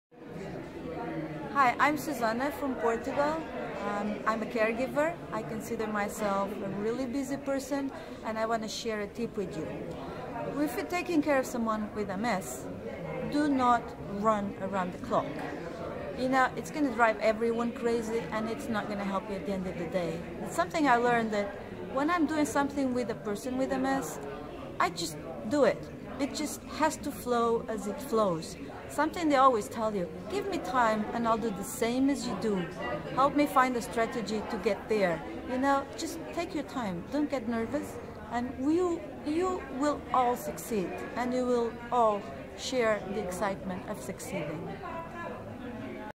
Susana's tip for caregivers
Susana shares a tip for caregivers of people living with MS. Share your tip using the LifewithMS hashtag!